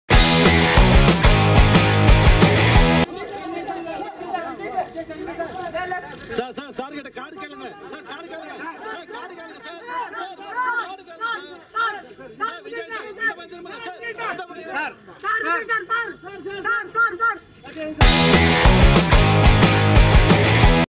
Ilayathalapathy Vijay entry in Nadigar Sangam Election Booth
An Entertainment You Tube Channel